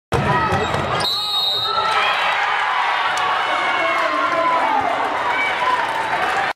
umcwbbvsCSP
Micaela Noga drives through the lane for the bucket and the foul for a big three point play